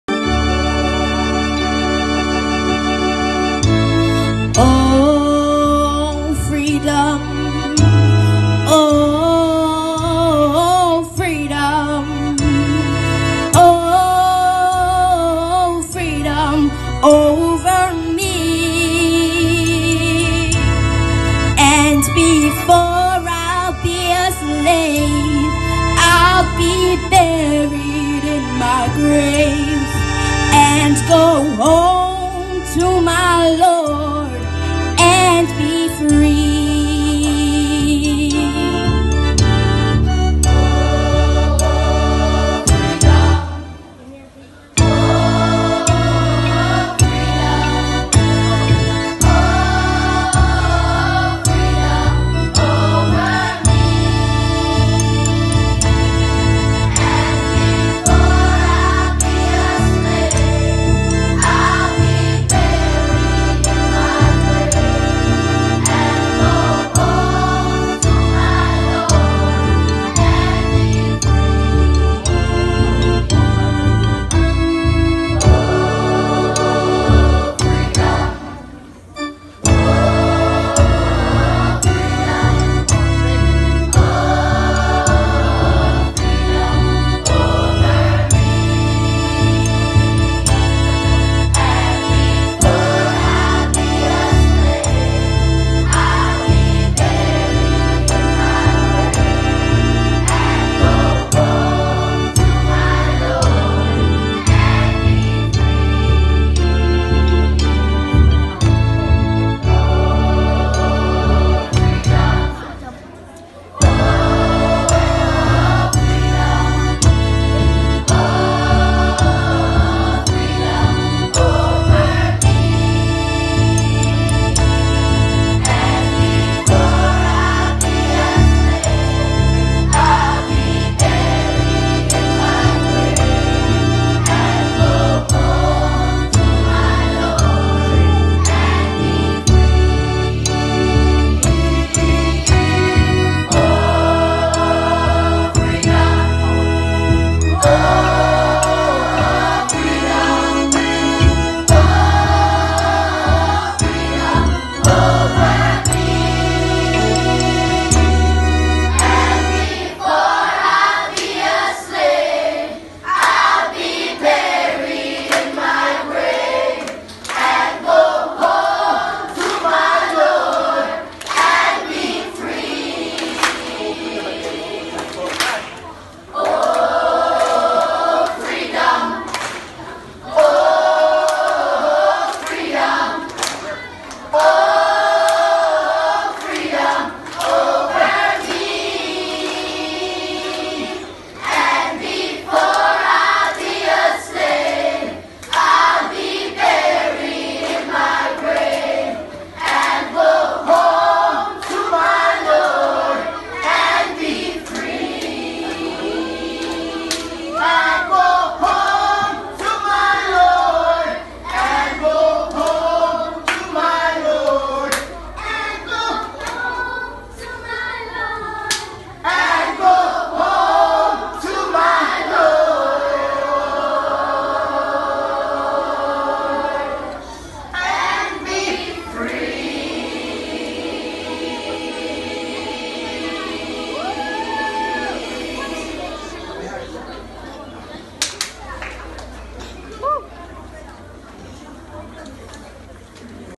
singing with my school choir .Black history celebration
Video from Howell